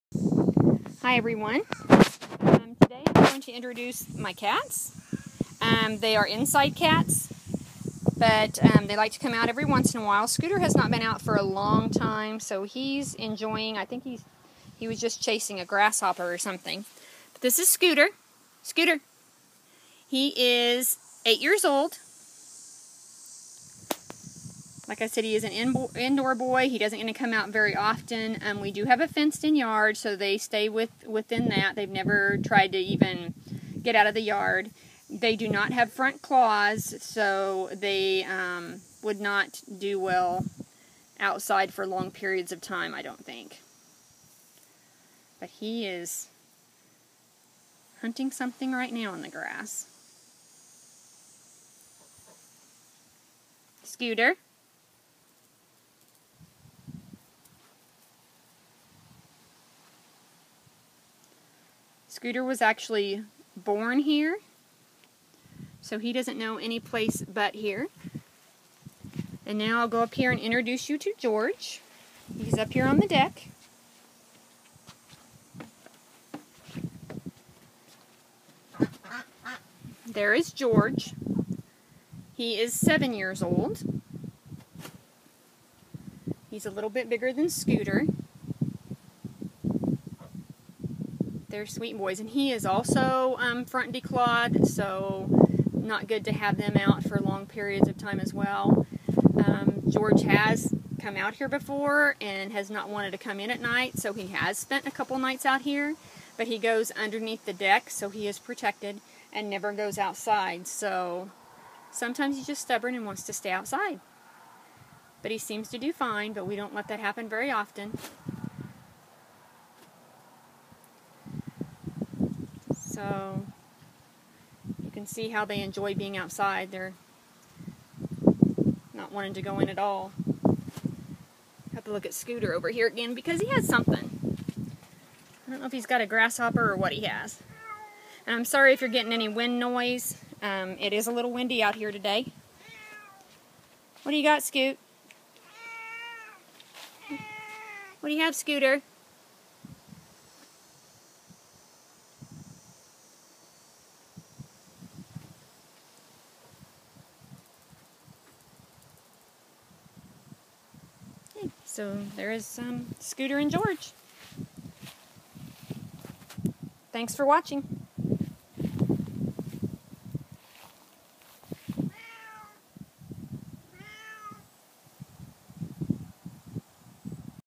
Introducing my Cats
Scooter and George enjoying the outdoors.